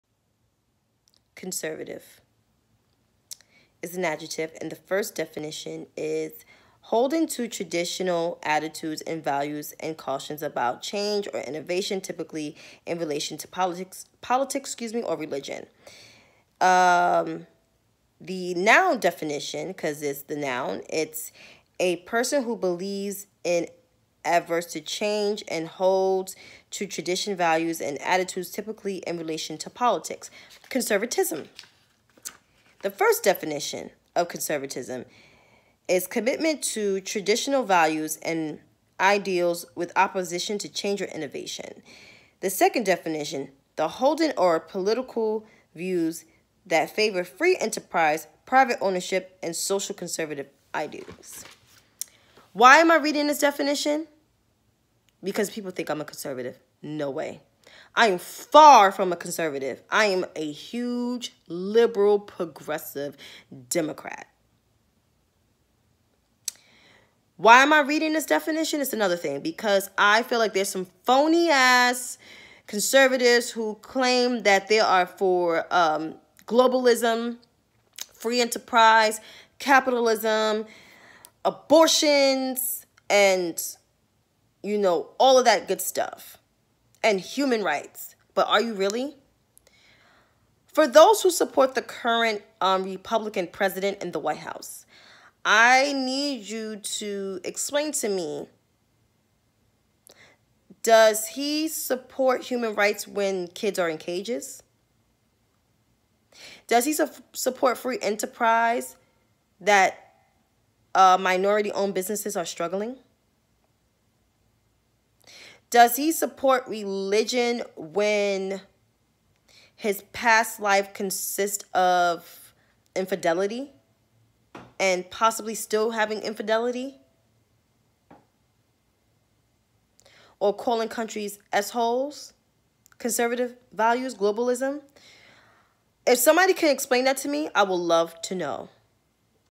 These Trump Supporters Are FAKE Conservatives
The meaning of conservatism and to be a conservative. Trump ain’t it.